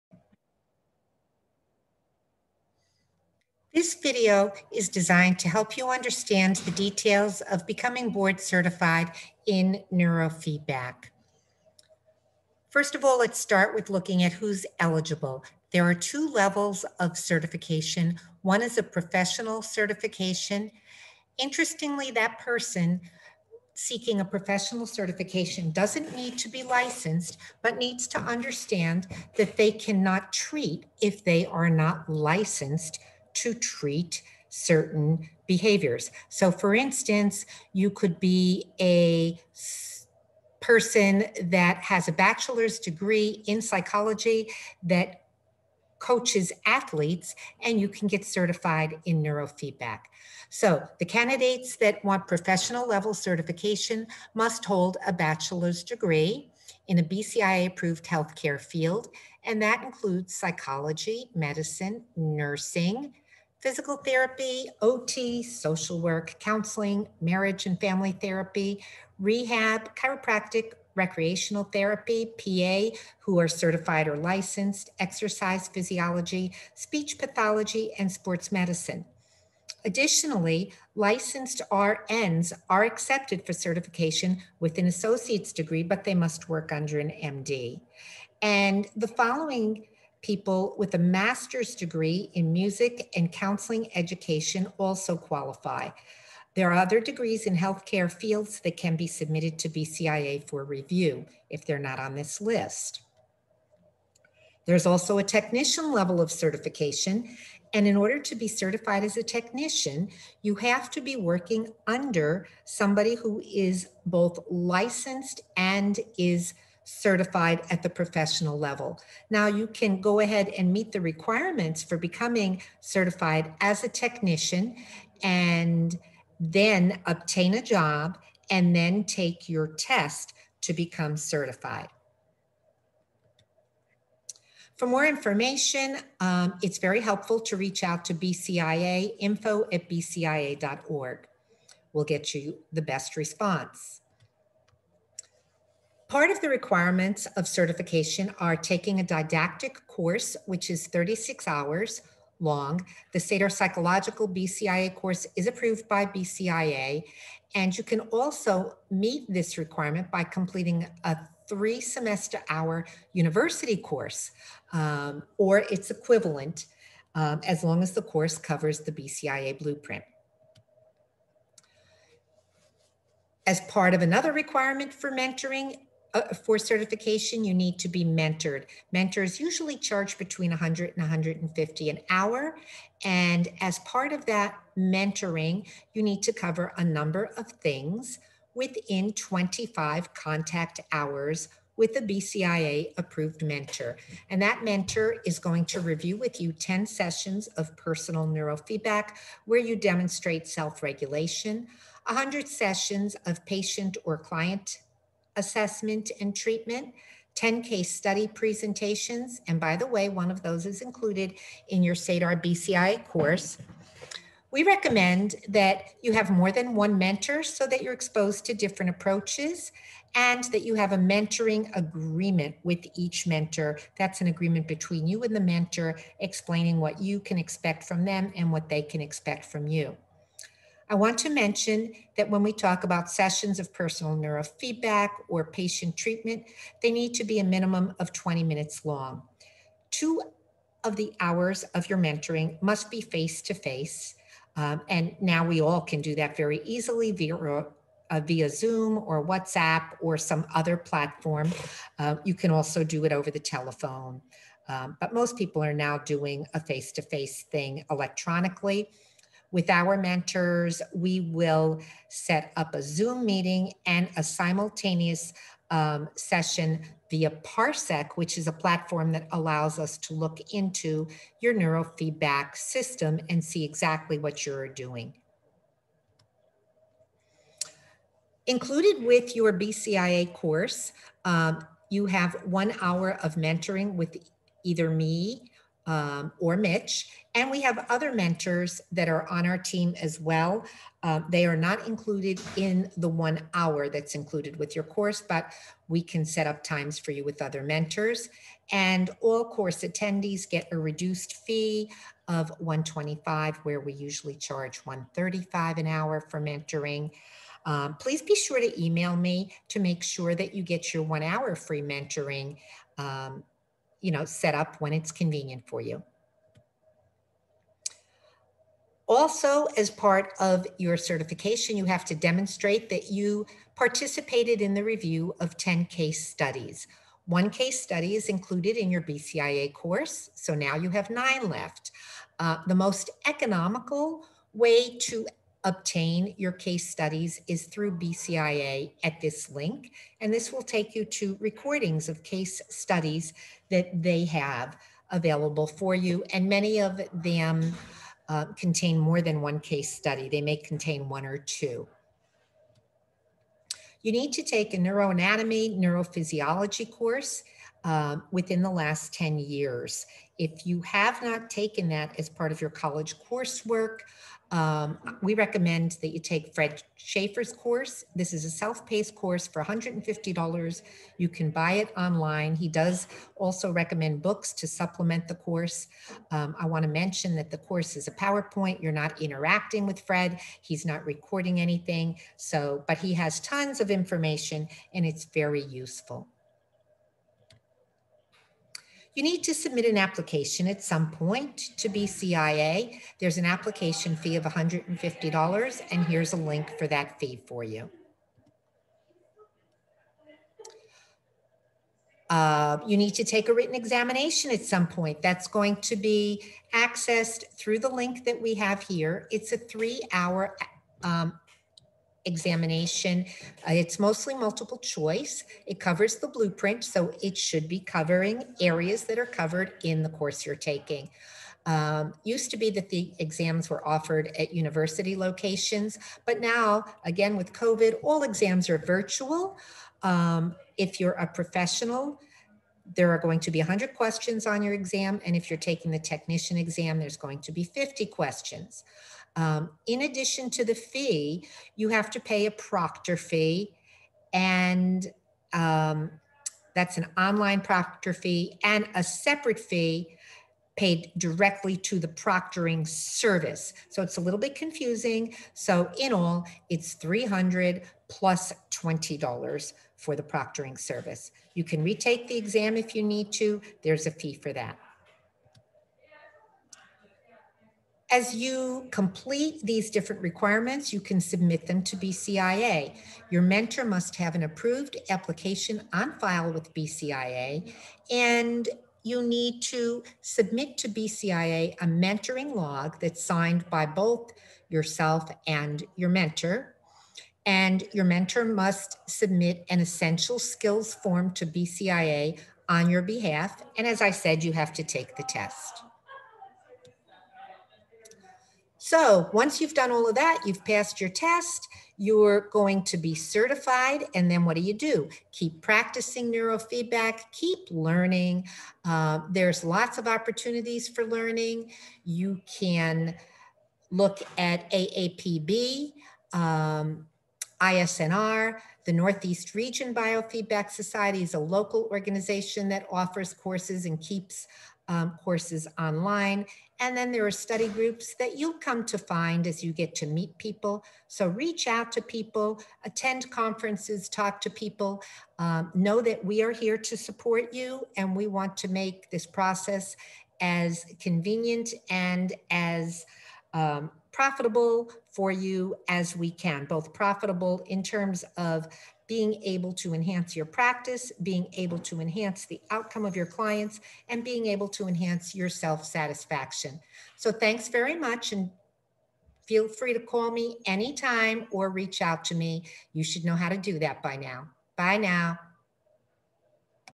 Details of Becoming BCIA Certified in Neurofeedback
Many people have questions about the qualifications and requirements to become BCIA certified.  In the Sadar Psychological and Sports Center BCIA course, we aim to help you become an accomplished neurofeedback provider.  In addition to offering a top notch course, we are here to help you throughout the certification process and beyond.